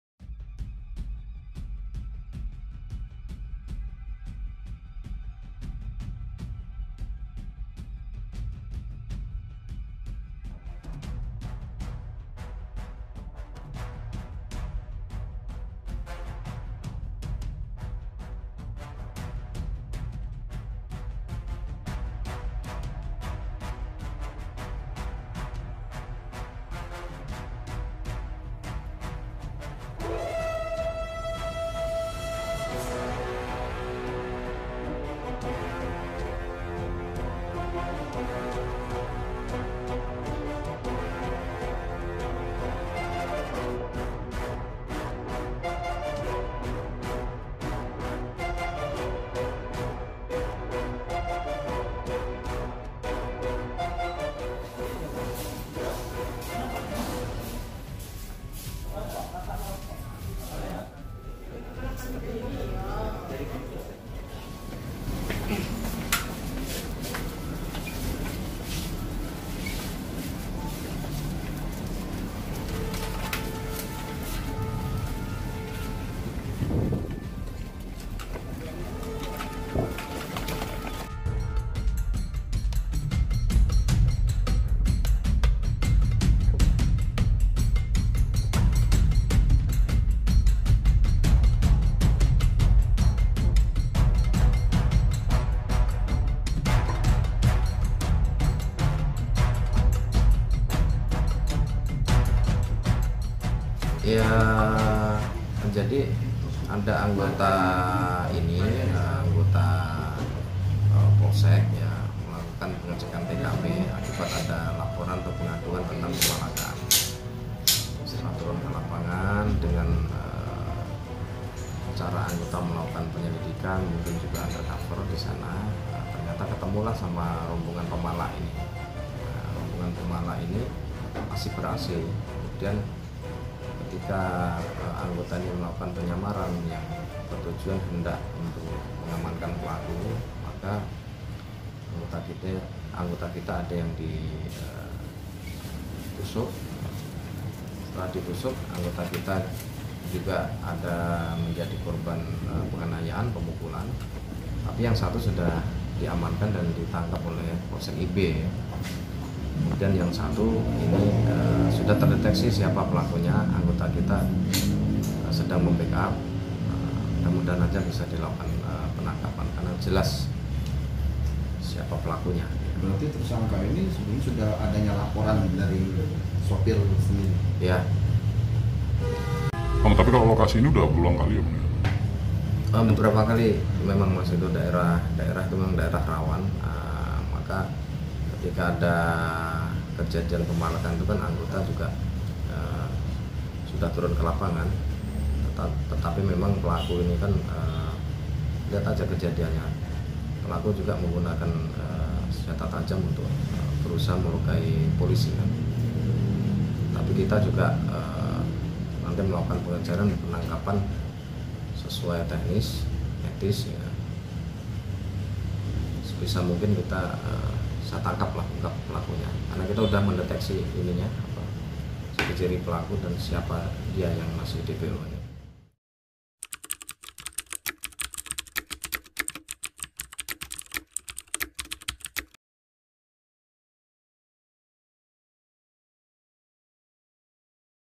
Pemalak Truk di Macan Lindungan, Polisi Malah Jadi Korban
Seorang polisi jadi korban pemalak di Simpang 4 Macan Lindungan Palembang. Seperti apa kejadiannya hingga polisi jadi korban??

Saksikan video di atas guys..